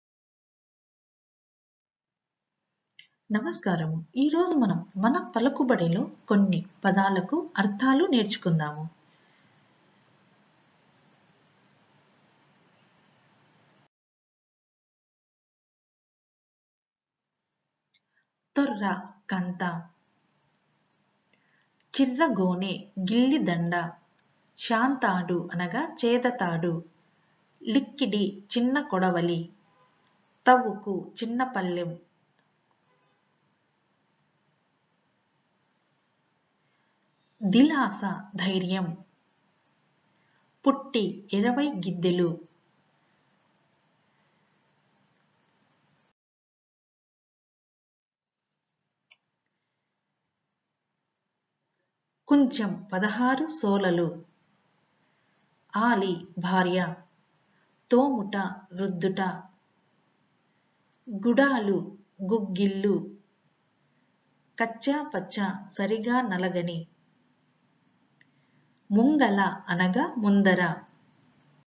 తెలుగు పలుకు బడులు - తెలుగు అర్థాలు / Telugu Meanings / Telugu Arthalu / Telugu Vyakaranam
ఈ వీడియోలో కొన్ని తెలుగు పలుకు బడులుకు వాటి తెలుగు అర్థాలు విందాము.